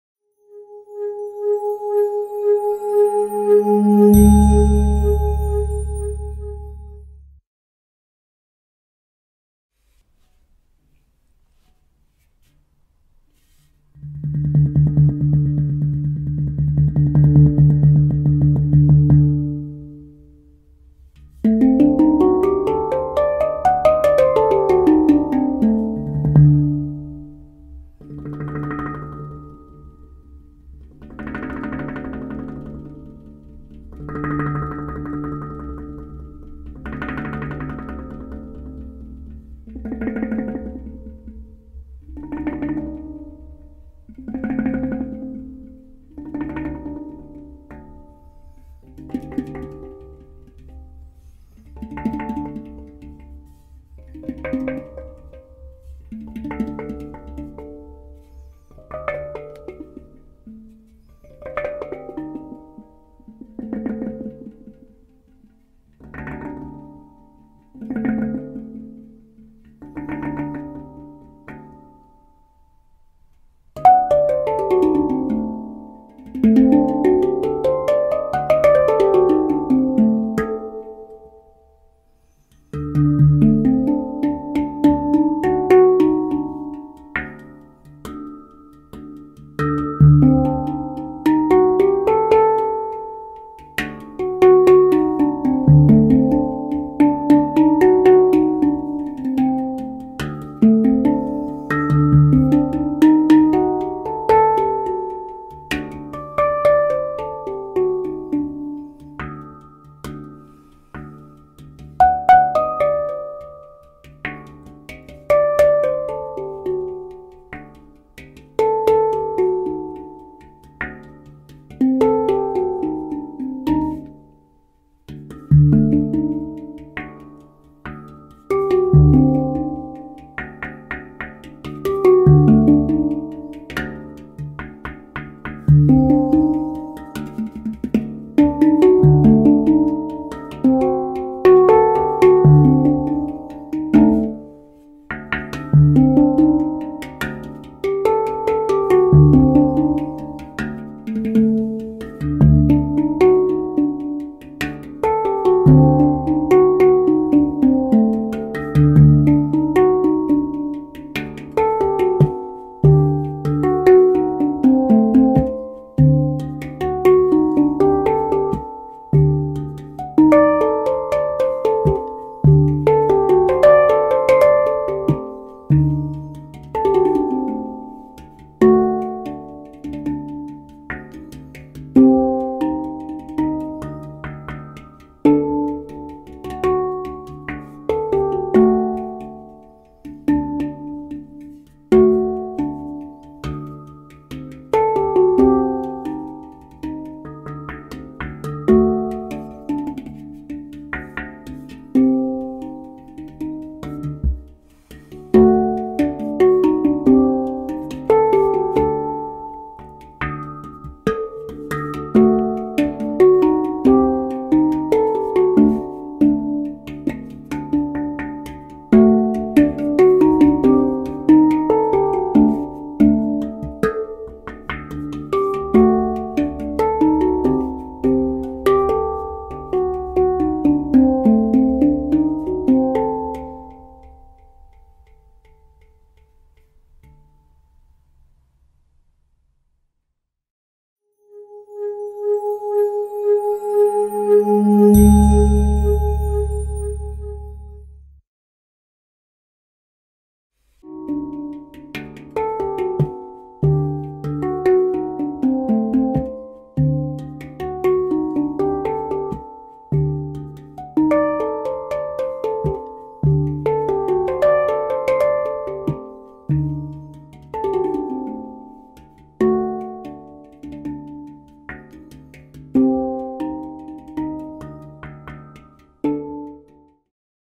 EchoSoundSculpture - AsaChan YuKon "YasmiNah" 12 Notes, 2020
EchoSoundSculpture - AsaChan "YasmiNah" D / A C D Eb F# G A C D Eb F#
A standard 53 cm size. 1.00 mm AsaChan Material. Headphones recommended. 720p HD for High Quality. Purchase AsaChan info